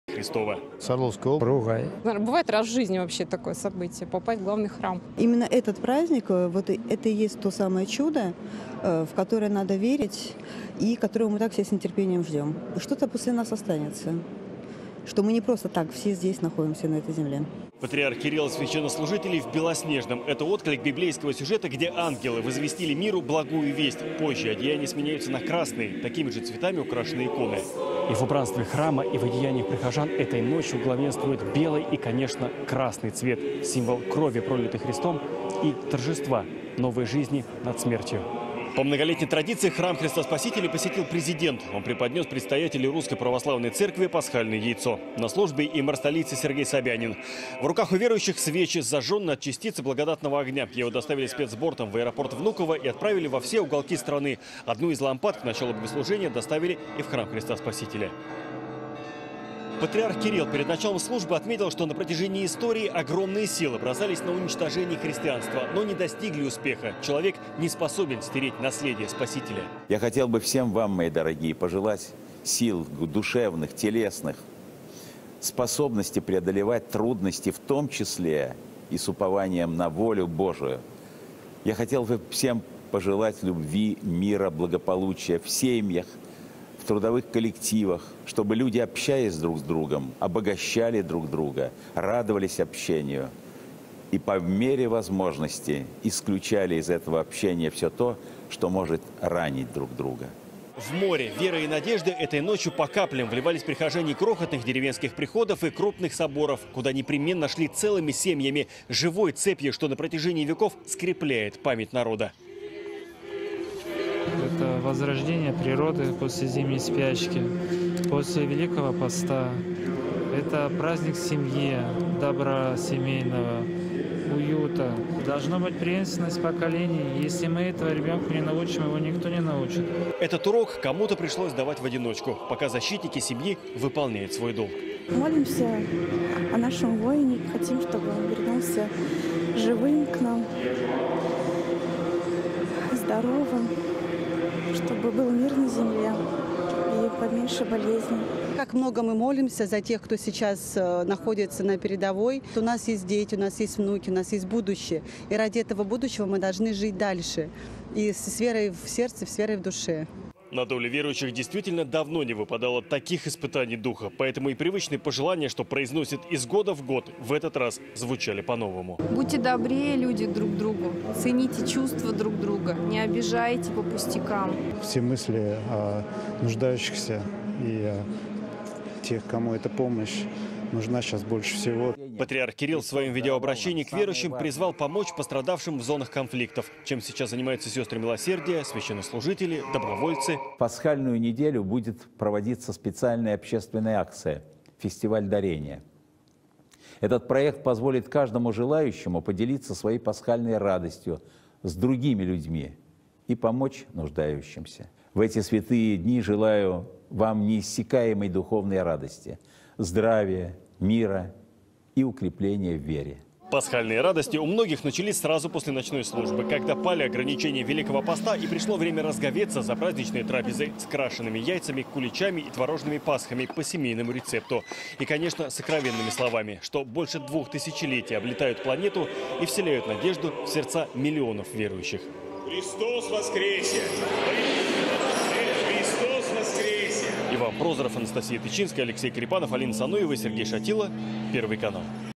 Пасха 2022. Служба в храмах и проповедь Патриарха
Пасха 2022. Служба в храмах и проповедь Патриарха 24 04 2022
Помощь каналу.